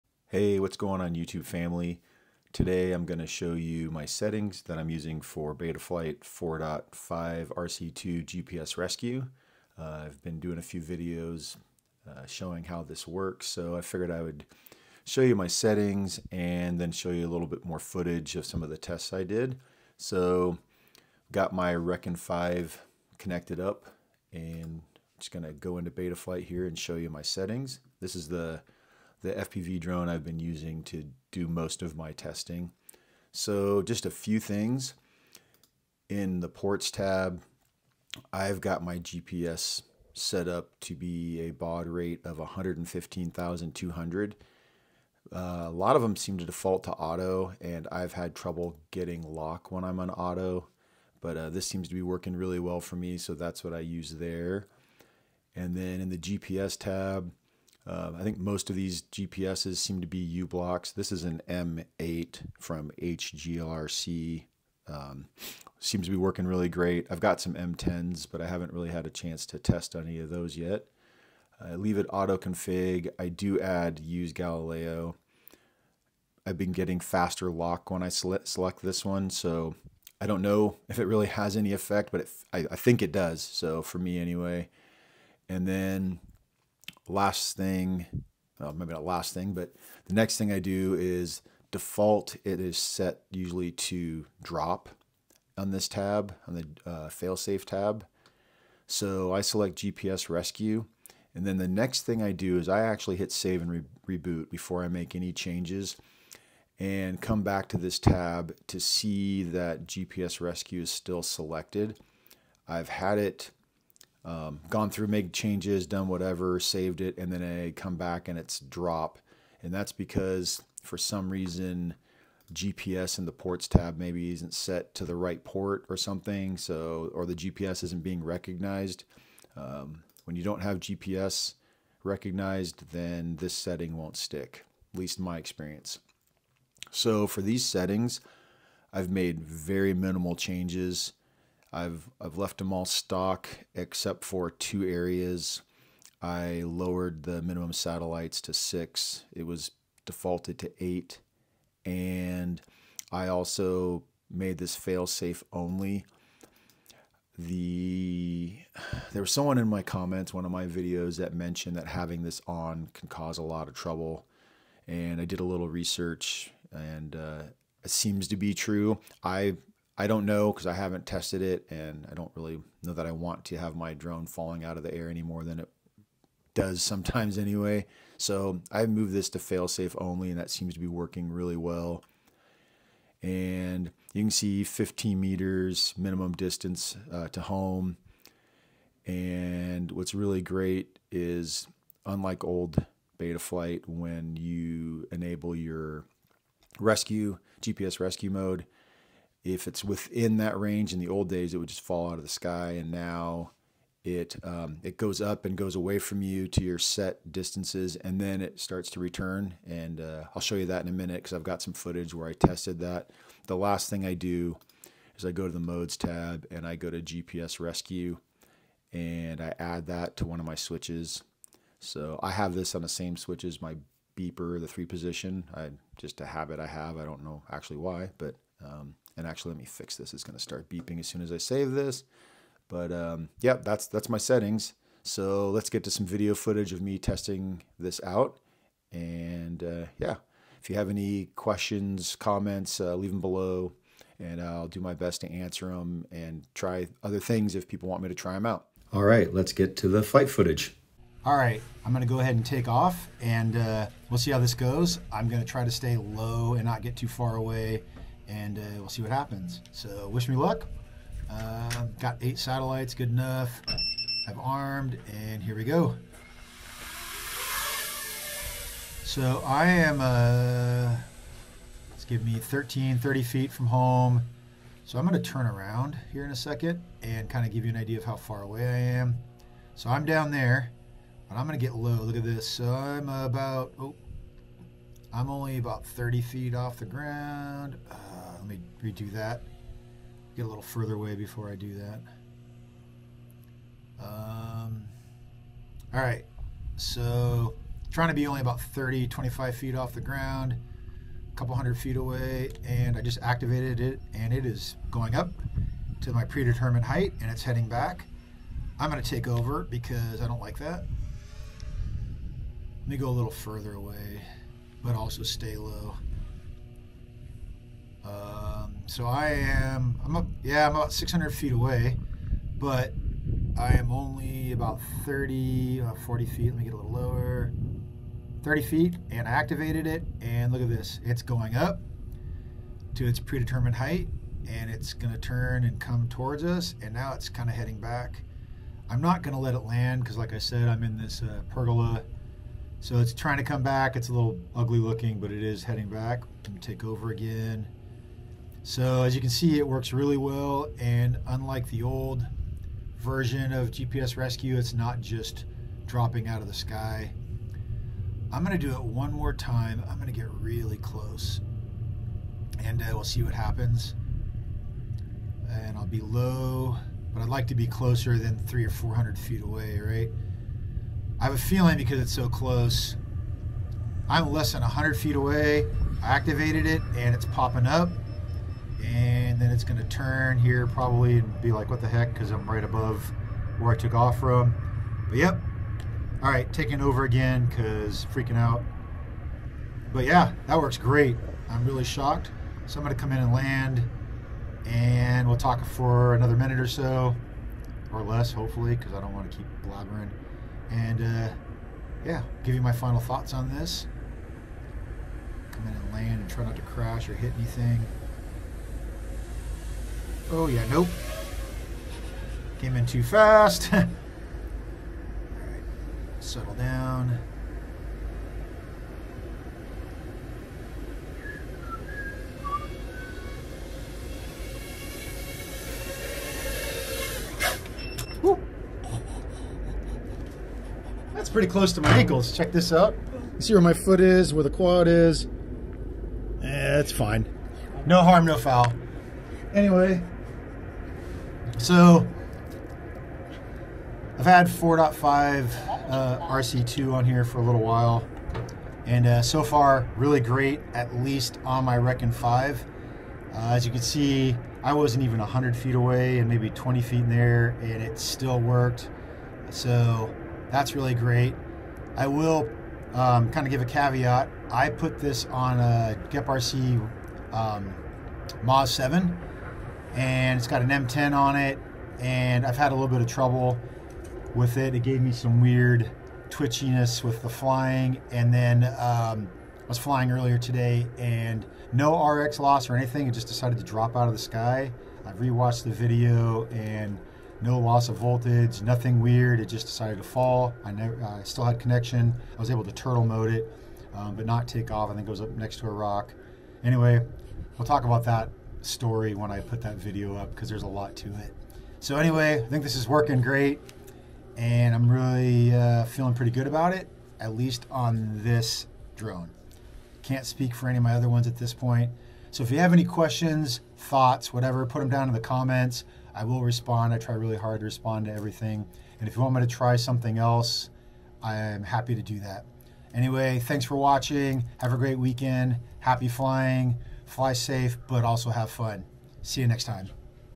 Betaflight 4.5 RC2 Return to Home Close Range/Low Altitude Testing
Testing Betaflight 4.5 RC2 return to home effectiveness while very close and low.

Drone:
Rekon5 FPV Long Range
Frame: Rekon5 LR
Wheelbase: 222mm
Arm thickness: 4mm
FC: Zeus F722 mini flight controller
ESC: ZEUS 28A 4in1 ESC blheli-s
VTX: DJI O3 Air Unit VTX
Motor: Rekon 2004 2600kv 4s
Propeller: GEMFAN 5125 3-blade propeller
GPS: M80 GPS
Buzzer: Soter buzzer
RX: 2.4G ELRS
Battery: CNHL 4S 1500 mAh lipo

Transmitter:
Radiomaster Pocket with HappyModel ES24TX Slim Pro ExpressLRS TX Module
Goggles:
DJI Goggles 2

Recording Equipment:
DJI Action 2 Camera
DJI MIC